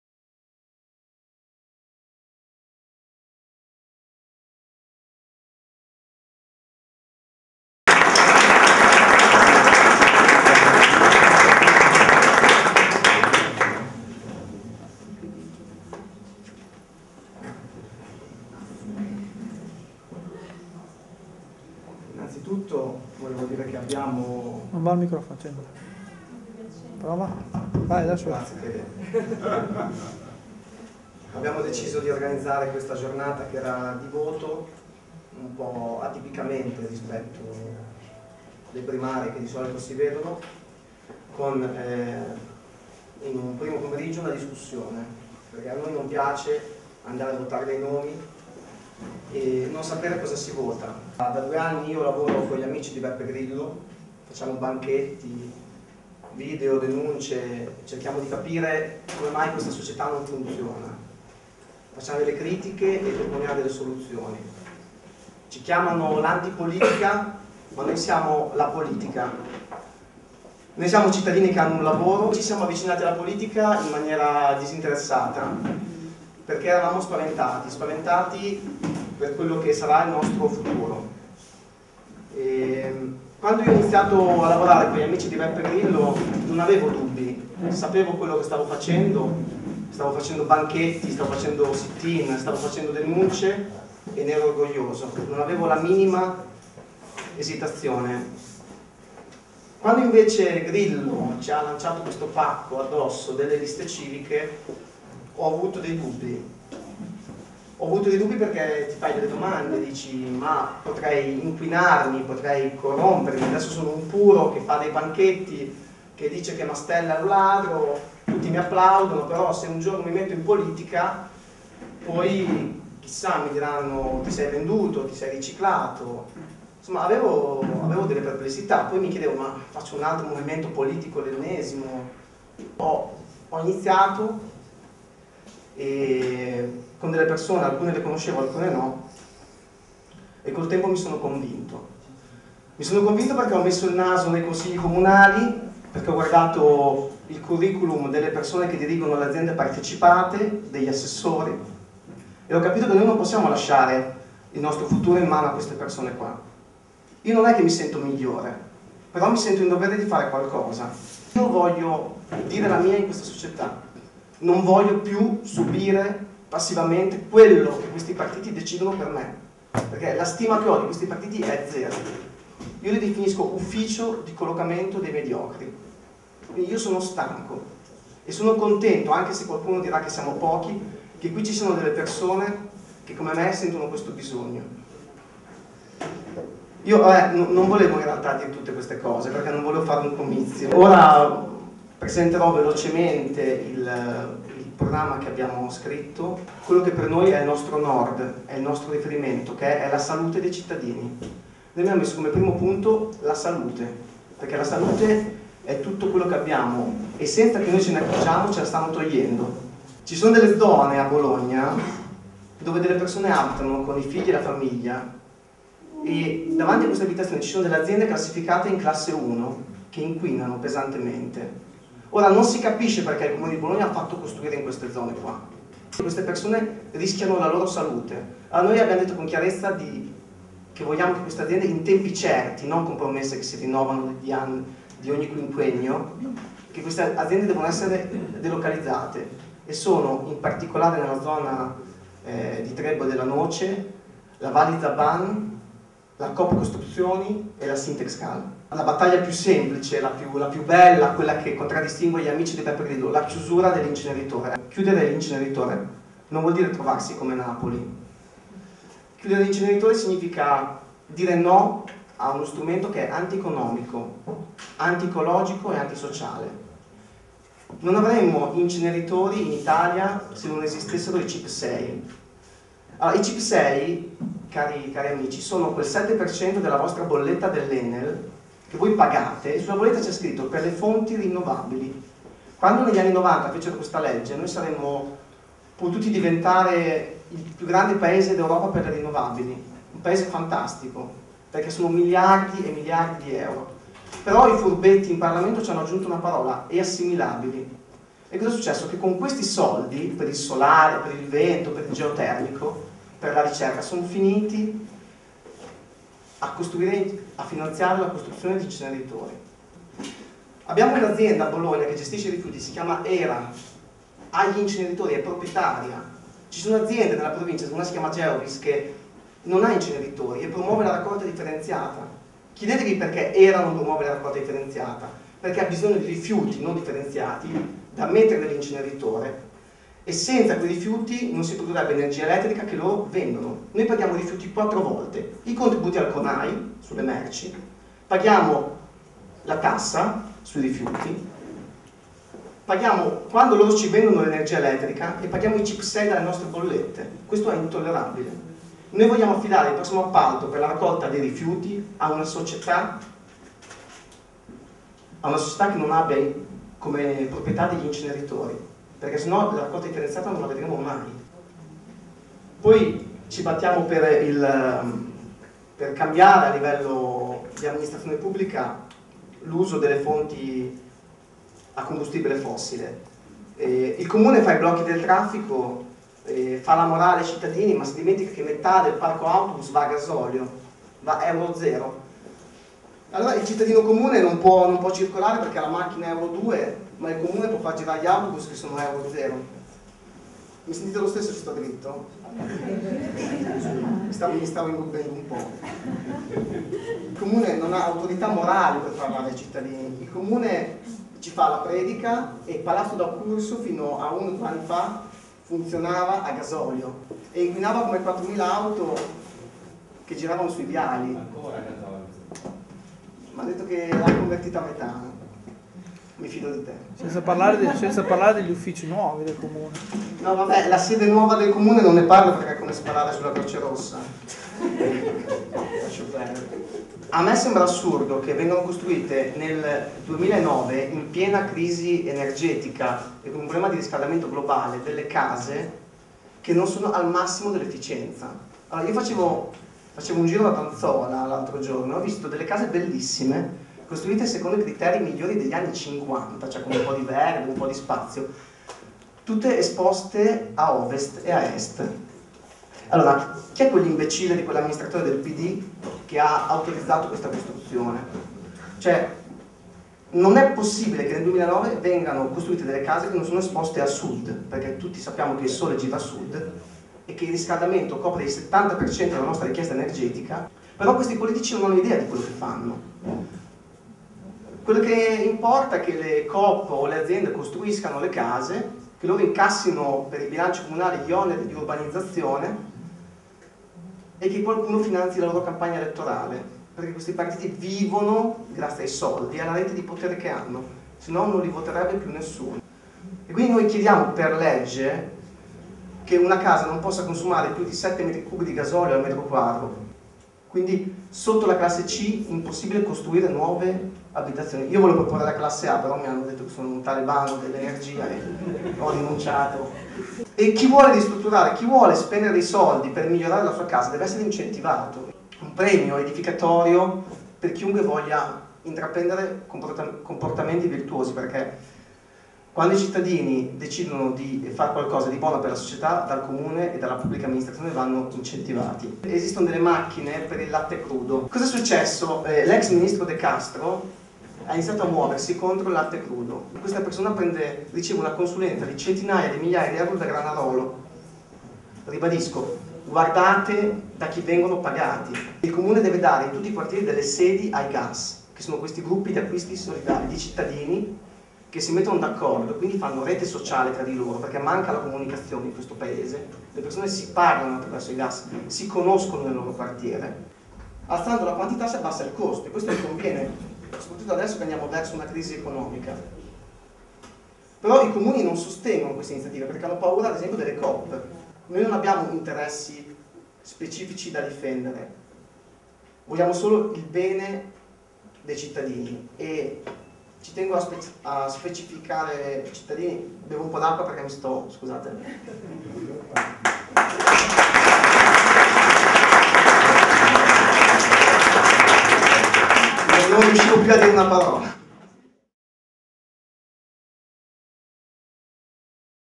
Giovanni Favia si candida a sindaco - prima parte
Il primo di Marzo gli Amici di Beppe Grillo di Bologna hanno organizzato un'assemblea, per scegliere la persona che dovrà rappresentarli all'interno delle istituzioni.
Riporto qui la prima parte del discorso con cui mi sono presentato. Parlare al di fuori delle nostre riunioni interne ad a una platea per lo più composta da sconosciuti, non era cosa di cui avessi pratica e confidenza.
L'emozione non è mancata... A breve pubblicherò anche la seconda parte.
Tenete le antenne alzate.

Giovanni Favia